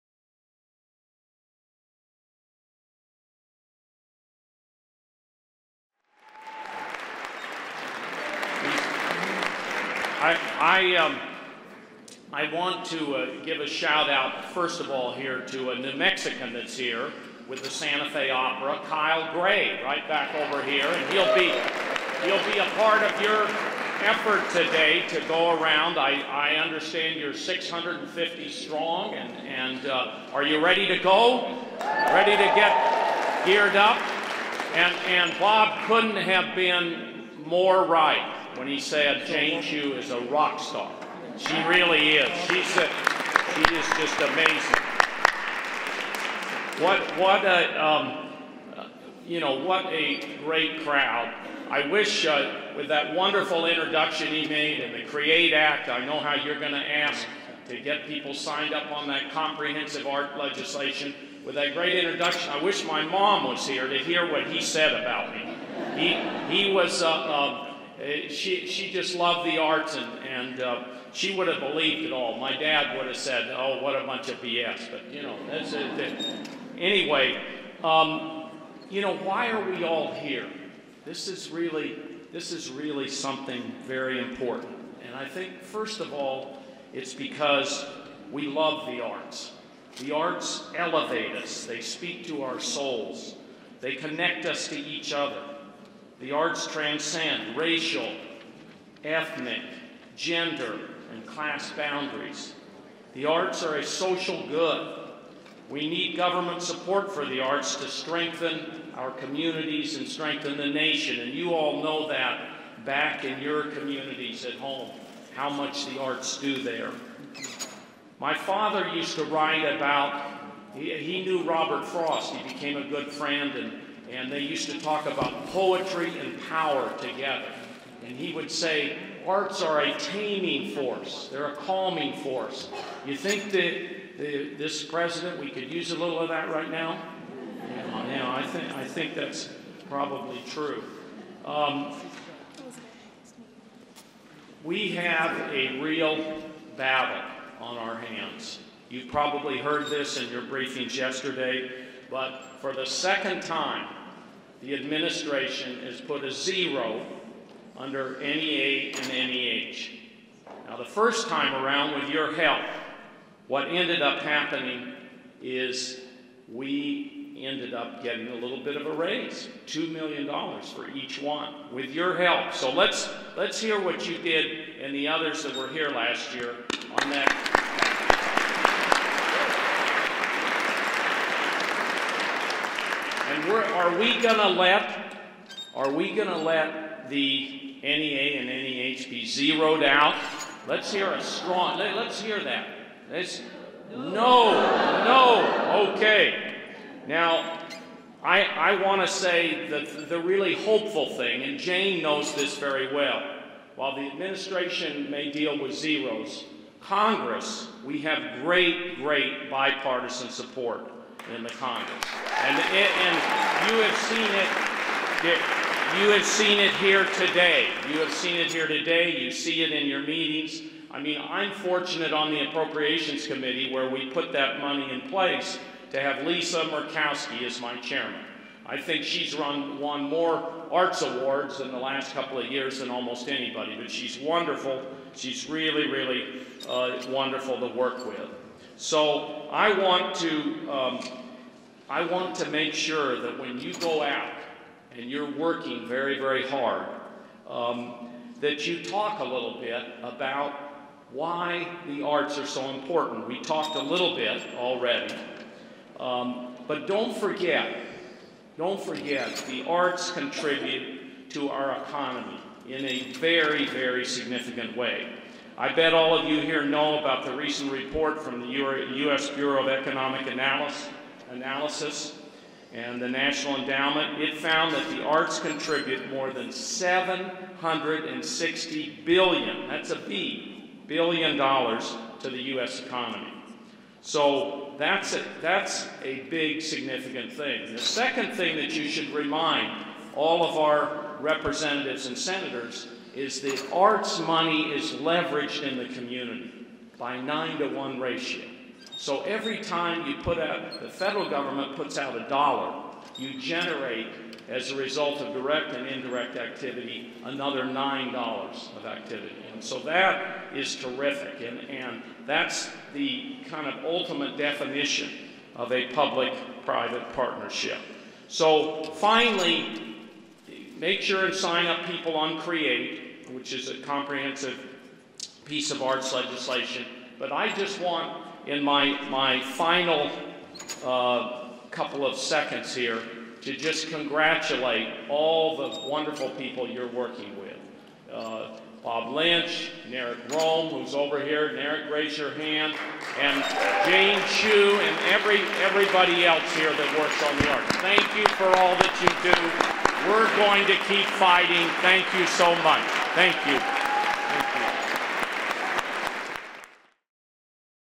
Arts Advocacy Day 2018: Senator Tom Udall
Senator Tom Udall (D-NM) speaks at Arts Advocacy Day 2018